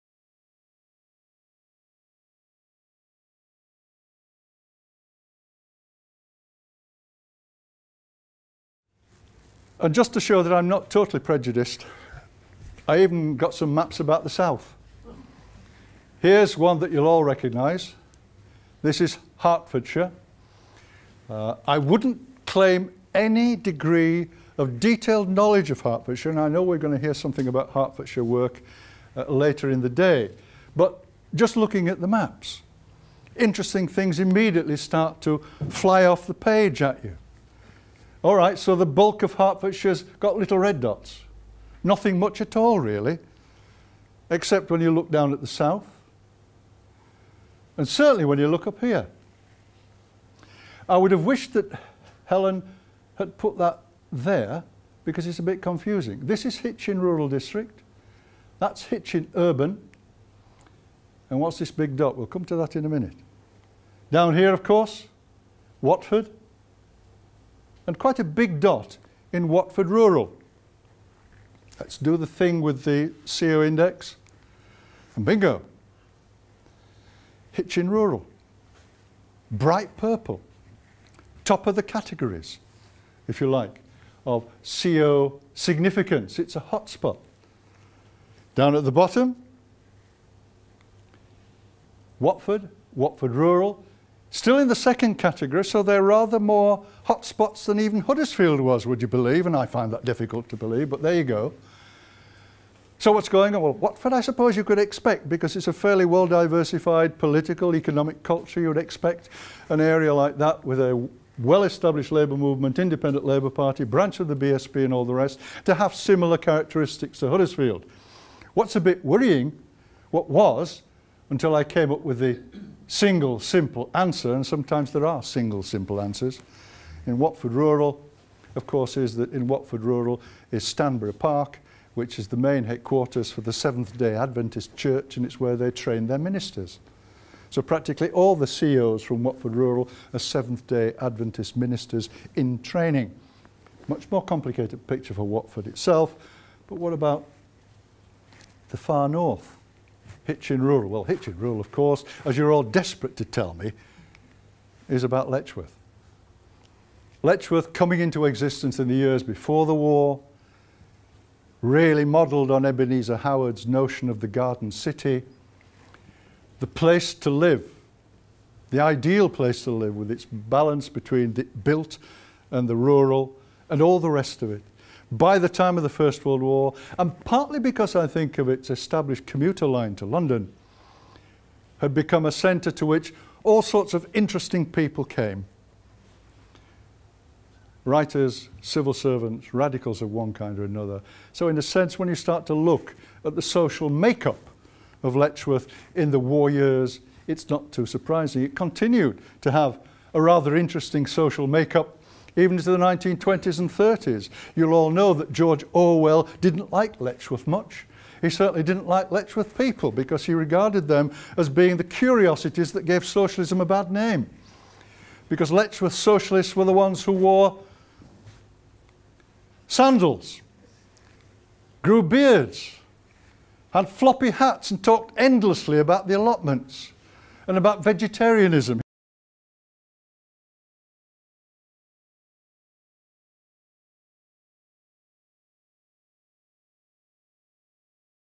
Cyril Pearce (University of Leeds) Clip 5
Conscientious Objection in Hertfordshire